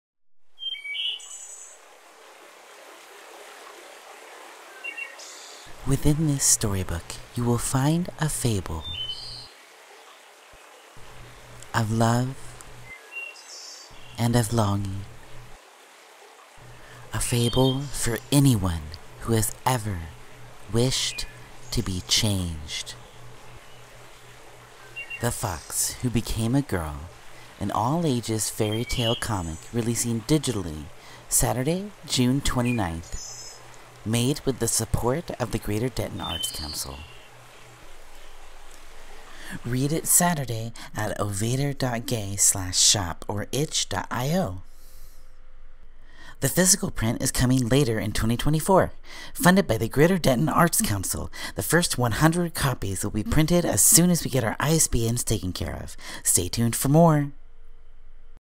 The Fox Who Became A Girl - DIGITAL RELEASE DATE TRAILER
The Fox Who Became A Girl was written by Chloe Bear and illustrated by Orion Fern

June 29th 2024 DIGITAL RELEASE

Available to read digitally 6/29 at
ovaettr.gay/shop
or
ovaettr.itch.io

Thanks for all your support for the comic!!!

PHYSICAL RELEASE later this year!